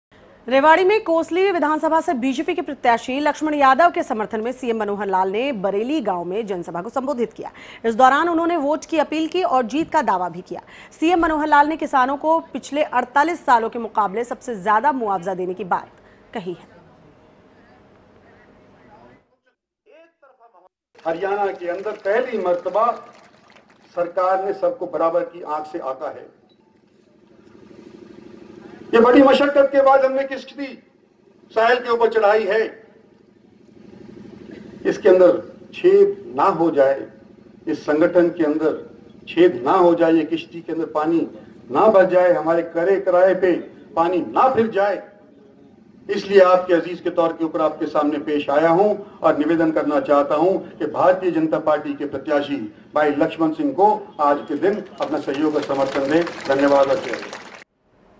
HARYANA: KOSLI में CM के साथ लक्ष्मण यादव ने किया प्रचार
रेवाड़ी में कोसली विधानसभा से बीजेपी प्रत्याशी लक्ष्मण यादव के समर्थन में सीएम मनोहर लाल ने बरेली गांव में जनसभा को संबोधित किया .. इस दौरान उन्होंने वोट की अपील की और जीत का दावा किया...